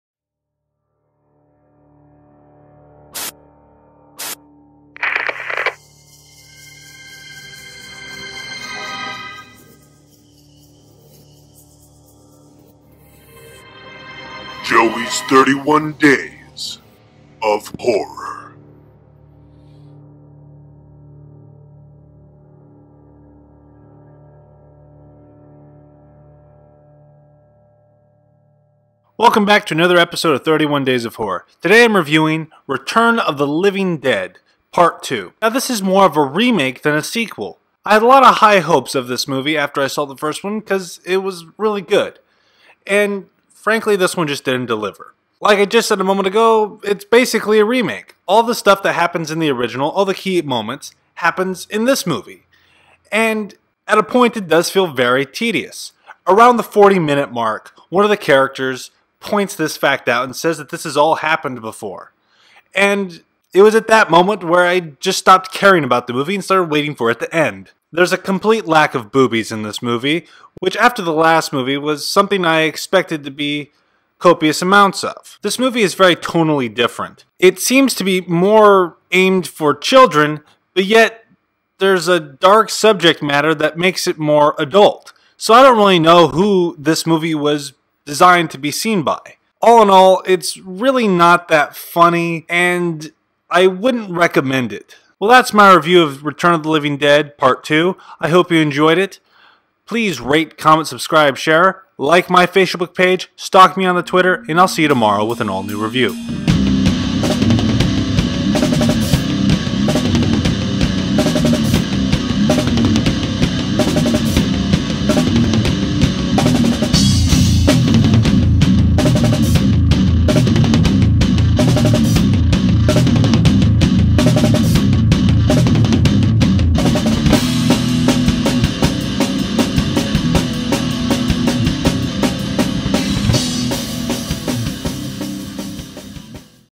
31 Days of Horror: Return of the Living Dead Part 2 (1988)
Day Sixteen: Return of the Living Dead Part 2 (1988)
Directed by Ken Wiederhorn
Staring: James Karen, Thom Mathews, Dana Ashbrook, and Marsha Dietlein